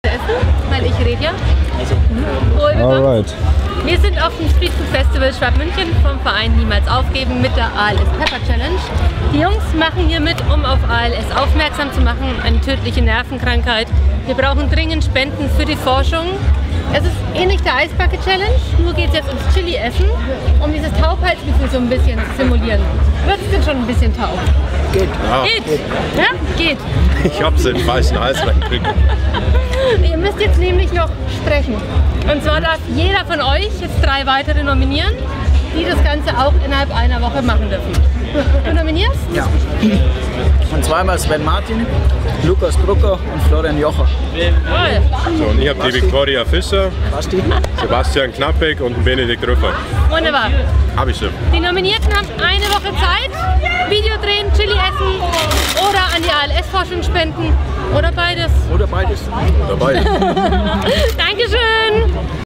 Live ALS Pepper Challenge auf dem Street Food SMÜ
Die ALS Pepper Challenge: Auch wenn die Challenge auf den ersten Blick wie Klamauk wirkt hat sie einen ernsten Hintergrund. Es wird auf die tödliche Krankheit ALS (Amyotrophe Lateralsklerose) aufmerksam gemacht und Spenden für die Forschung gesammelt. Der Nominierte hat eine Woche Zeit selber ein Video zu drehen und drei weitere zu nominieren. Macht er nicht mit "muss" er an die ALS Forschung spenden. Verbreitet wird die Aktion derzeit vom gemeinnützigen Verein niemALS aufgeben e.V. die nach einem Jahr allein durch die Challenge knapp 6,5 Tausend EUR für die Grundlagenforschung der ALS spenden konnte.
Das Video wurde im Mai 2019 auf dem Street Food Festival in Schwabmünchen aufgenommen.